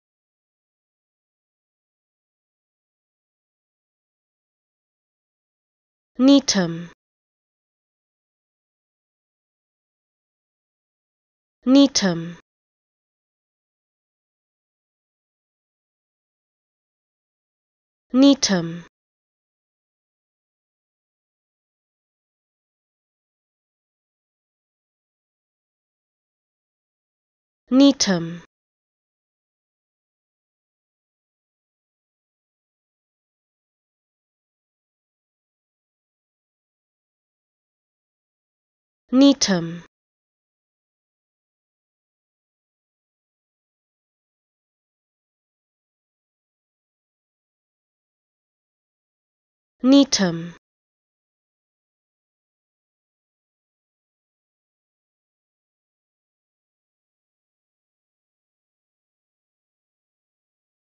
How To Say Gnetum
Learn how to say Gnetum with EmmaSaying free pronunciation tutorials.